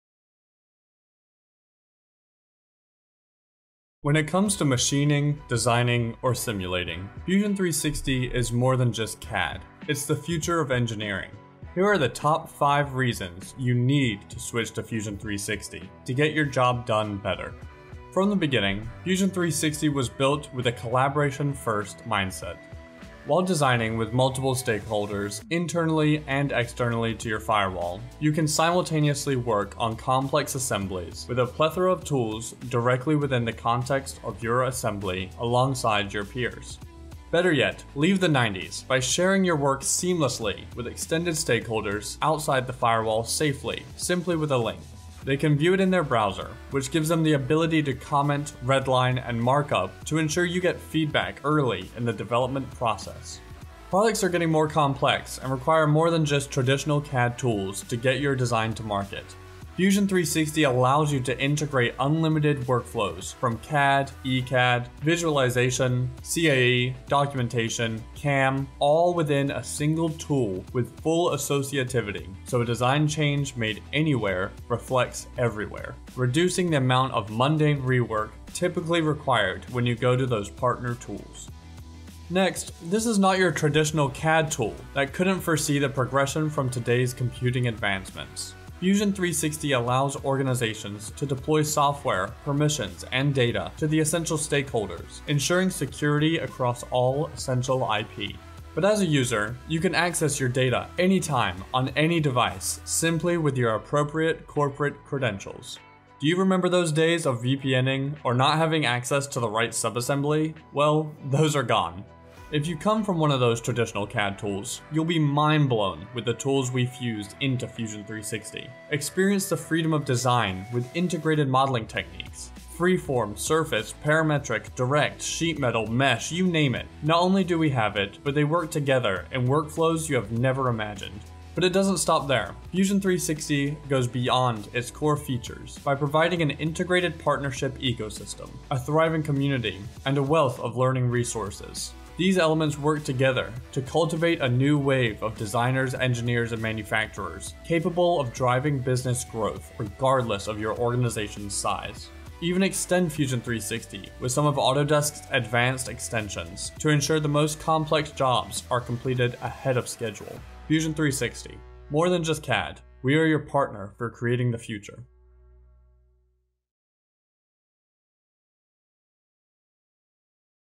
Top 5 Reasons to Switch to Autodesk Fusion 360
Looking to switch to Fusion 360 for your design and engineering needs? Discover the top 5 reasons why Fusion 360 is the ultimate choice. From data and collaboration to modern CAD tools, this video covers it all.

1. Data and Collaboration at the Center: Experience seamless collaboration with Fusion 360's cloud-based platform. Share design data and communicate effortlessly with team members, clients, and suppliers. Collaboration has never been easier.

2. One Product, Unlimited Possibilities: Unlock unlimited potential with Fusion 360's all-in-one solution. From concept to manufacturing, Fusion 360 offers a comprehensive suite of tools to streamline your entire product development process.

3. Your Gateway to Secure, Anytime Access: Enjoy secure, anytime access to your designs with Fusion 360's cloud storage. Work from anywhere, on any device, and never miss a beat.

4. Modern CAD and 3D Modeling Tools: Explore Fusion 360's cutting-edge CAD and 3D modeling capabilities. From parametric modeling to organic sculpting, Fusion 360 provides the tools to bring your ideas to life. Stay at the forefront of design innovation.

5. Customizable Advanced Functionality: Tailor Fusion 360 to your unique needs with customizable advanced functionality. Enhance your workflow with plugins and add-ons, and create a personalized design experience.

Switching to Fusion 360 is a game-changer for designers and engineers. Discover why millions of users have already embraced this powerful tool. From data-driven collaboration to comprehensive design capabilities, Fusion 360 has it all.

If you want more information on why Fusion 360 is the go-to solution for your design needs, check out our blog post

Don't miss out on this opportunity to elevate your workflow and unleash your creativity.